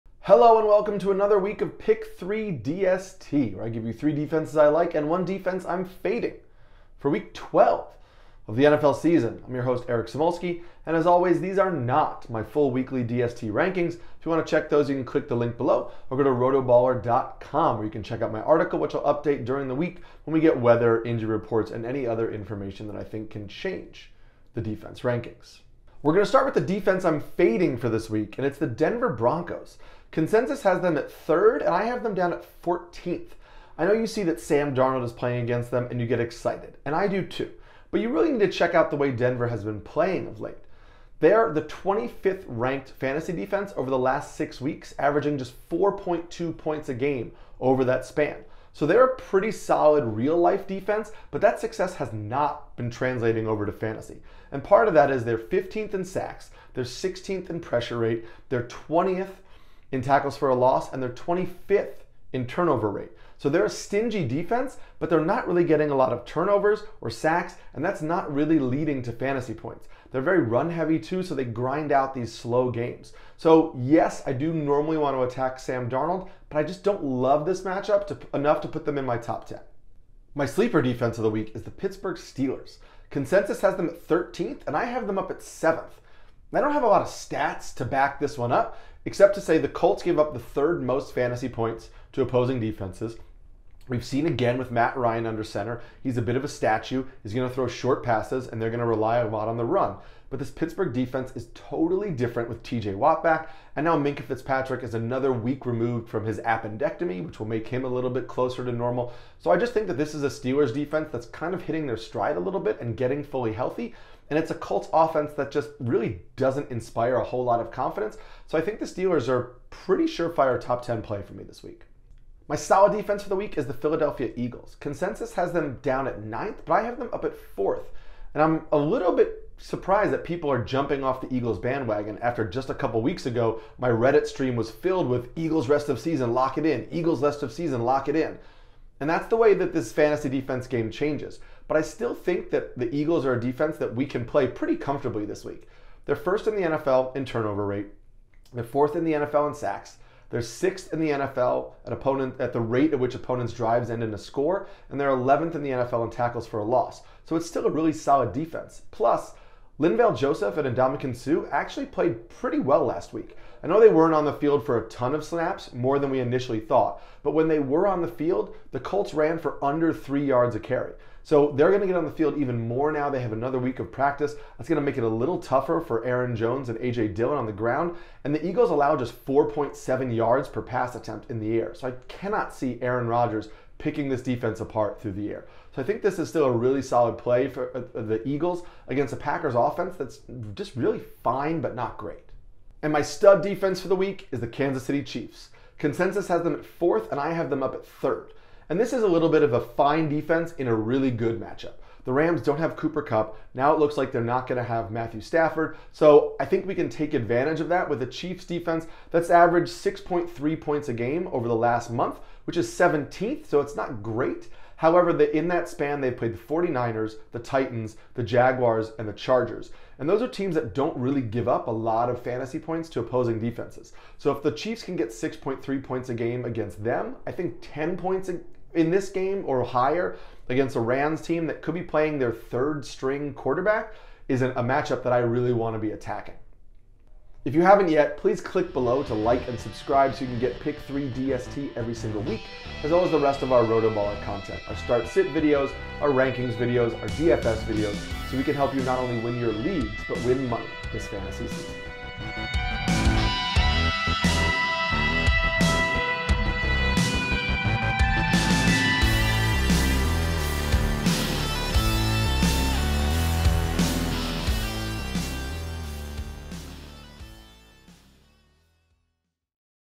Defense (D/ST) Start Em / Sit Em - Fantasy Football Week 12
Eric Samulski's (@SamskiNYC) team defense (D/ST) TOP PLAYS for fantasy football Week 12 of the 2022 NFL season. He gives his must-start and must-sit DEF options in Week 12 to help fantasy football managers make tough lineup decisions at defense (D/ST).

You can read his full Week 12 DST Rankings and Waiver Wire Streamers article here with tiered rankings and analysis each week